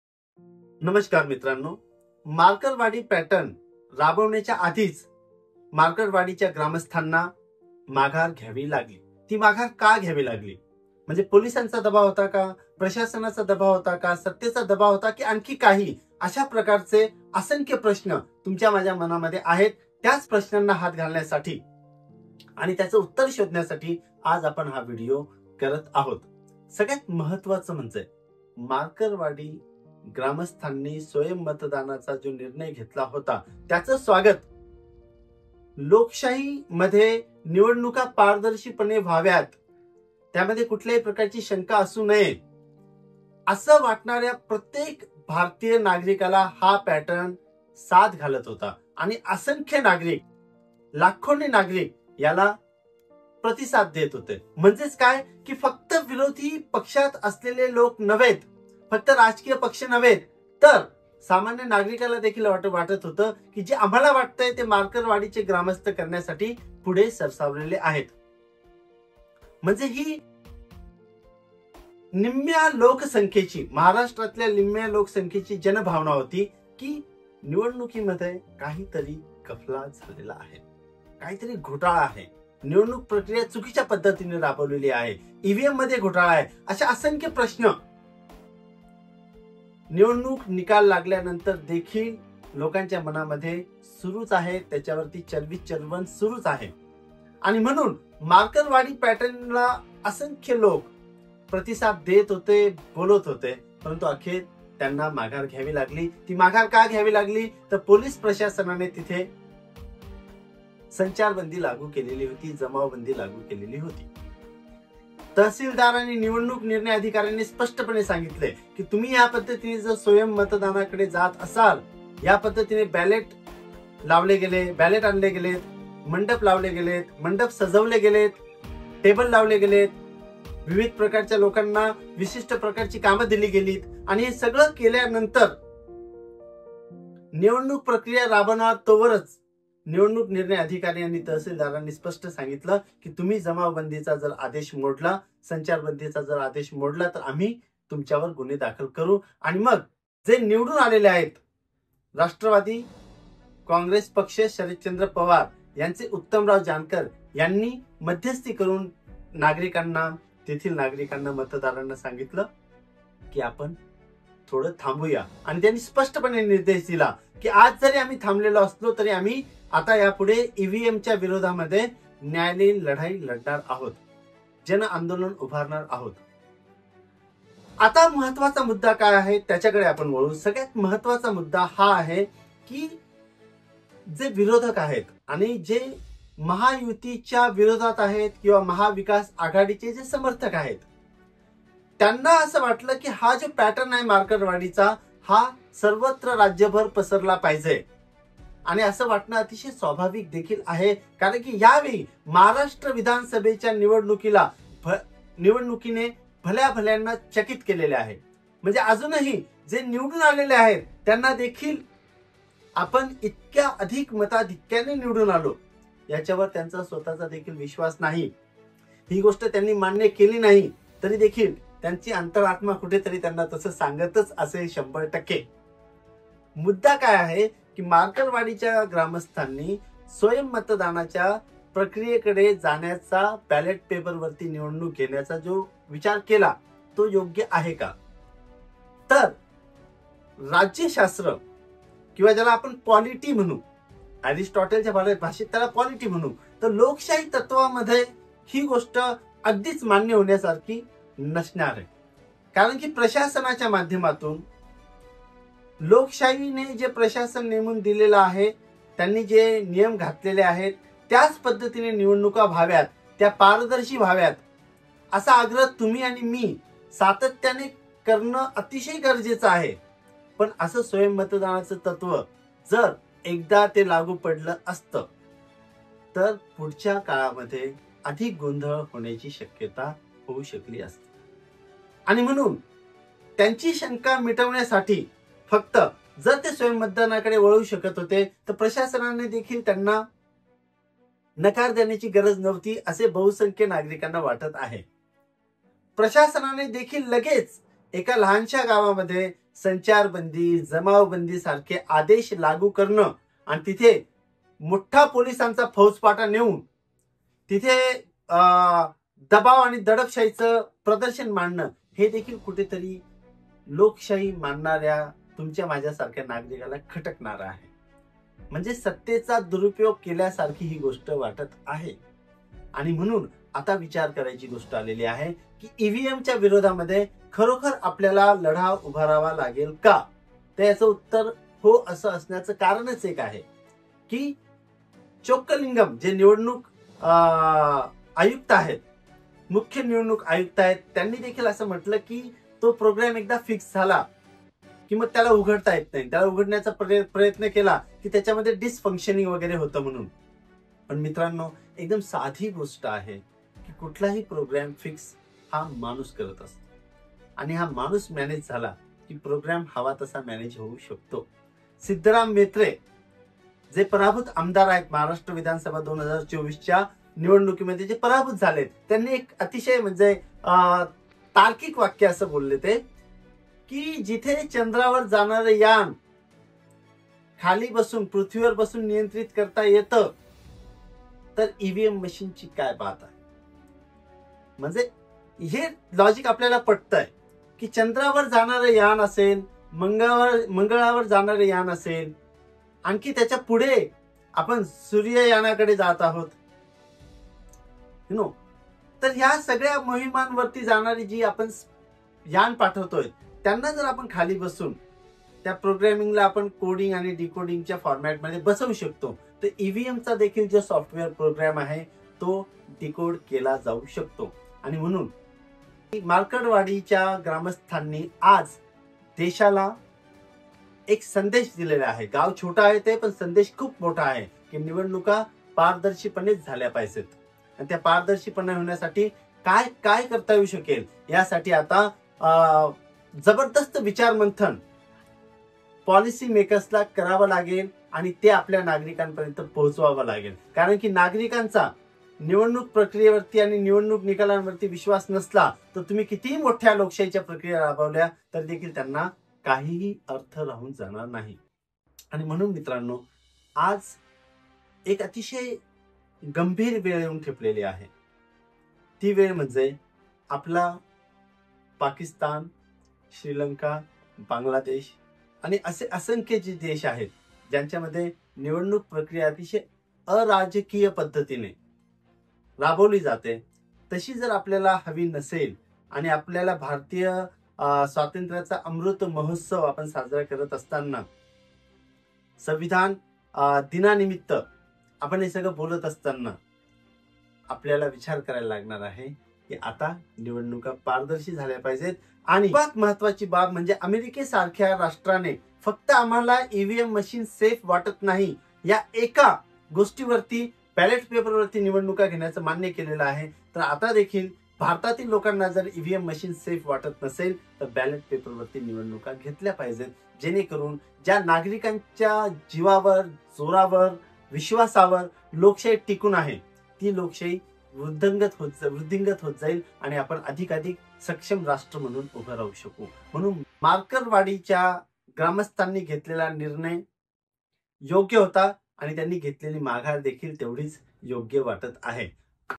Marakadwadi Ballot Paper Voting Update| मारकडवाडी ग्रामस्थांनी माघार का घेतली ? | राज सर
सोलापूर मधील मारकडवाडी ग्रामस्थांची अखेर माघार घेतली. गावात काय घडलं? एकही मत न टाकता मतदान प्रक्रिया थांबवण्यात आली. या मागे नेमका पोलिसांचा दबाव , प्रशासन की आणखी काही कारण आहेत याविषयी या वीडियो मध्ये चर्चा करण्यात आलेली आहे .